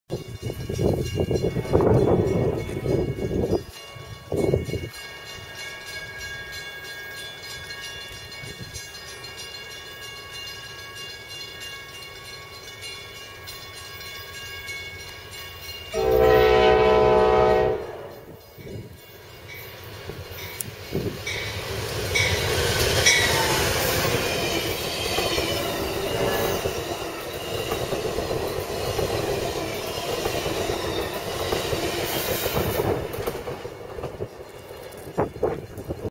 Amtrak 56 West - Northbrook, IL
Amtrak P42 56 leads a Hiawatha west through Northbrook.